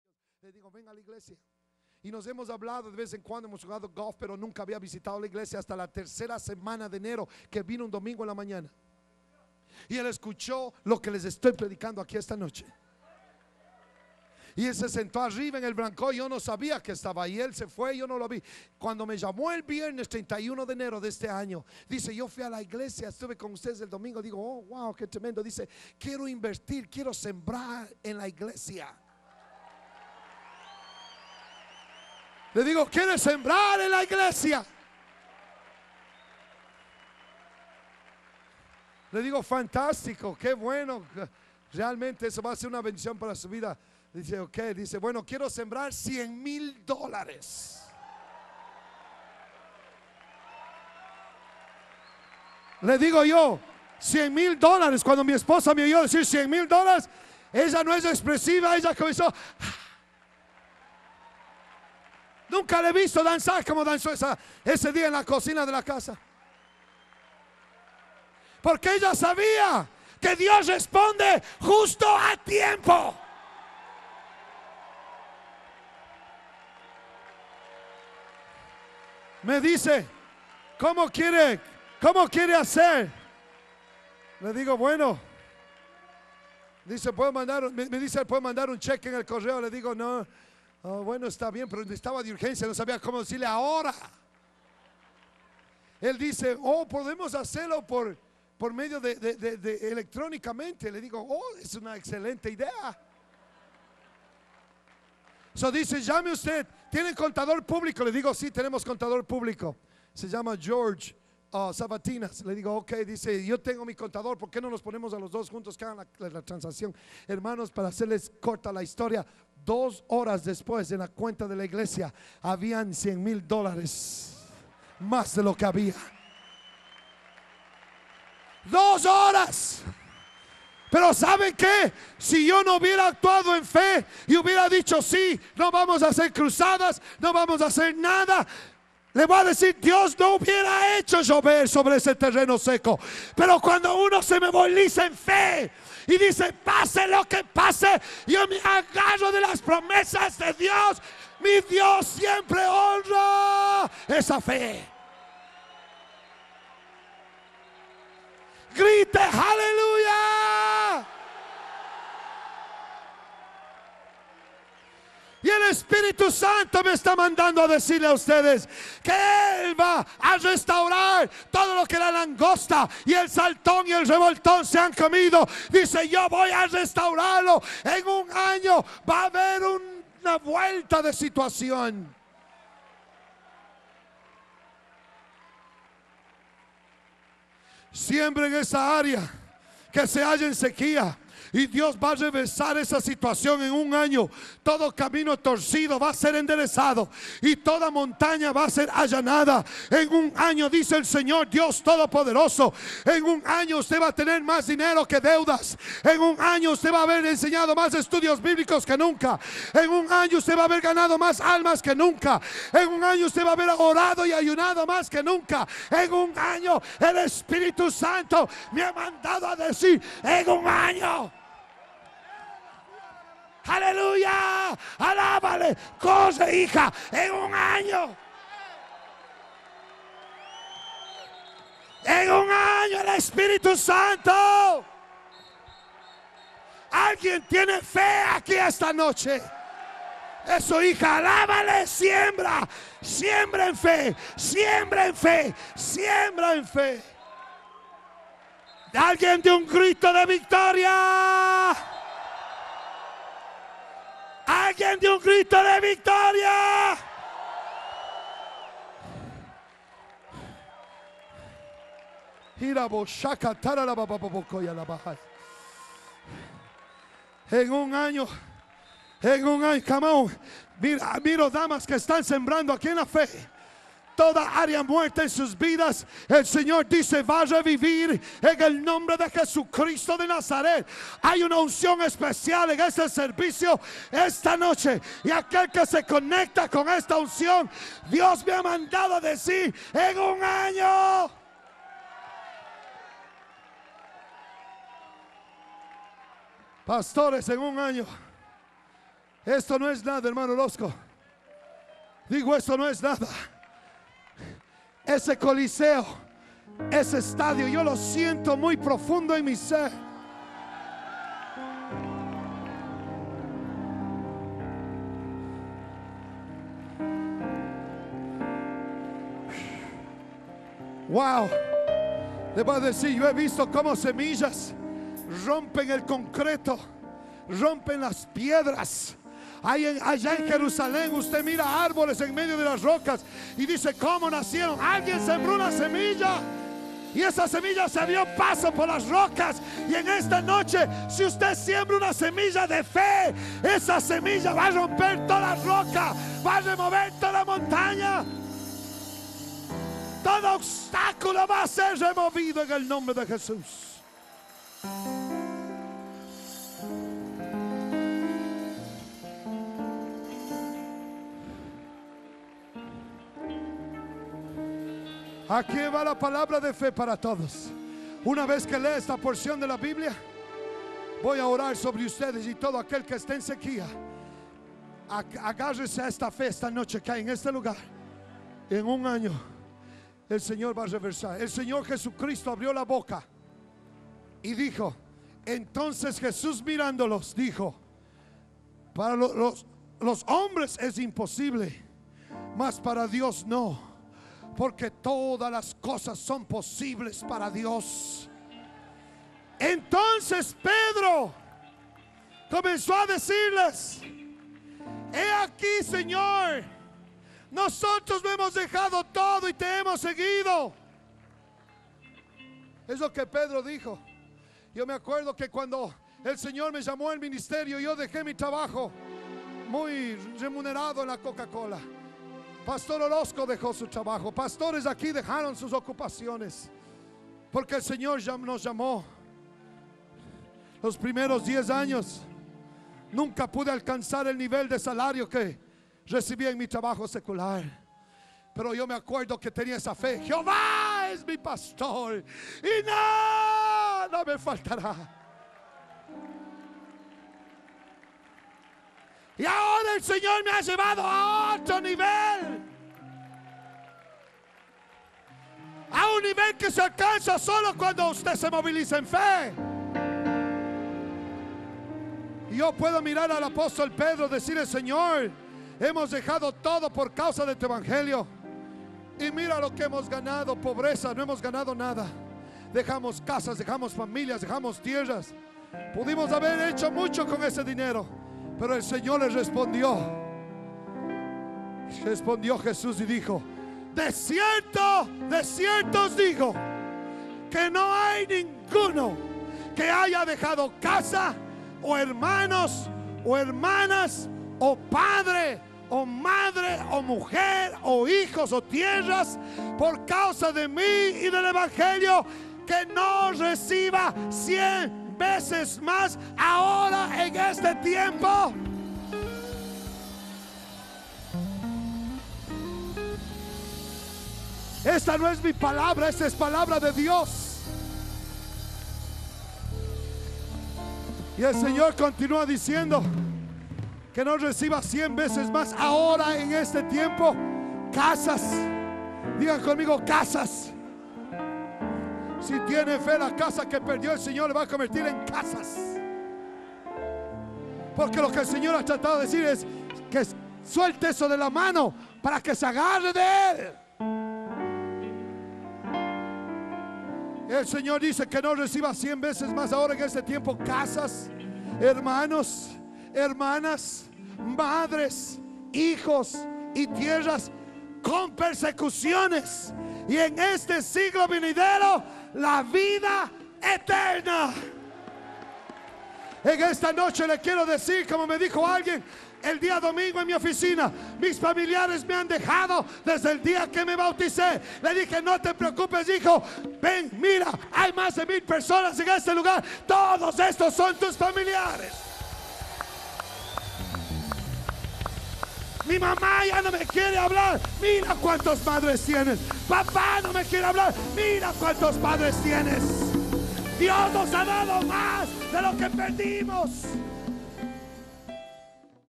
Rev. Elias Limones - Tiempo De Cosecha 2008
Preaching: Rev. Elias Limones
On the importance of Sacrificing even in times of hardship.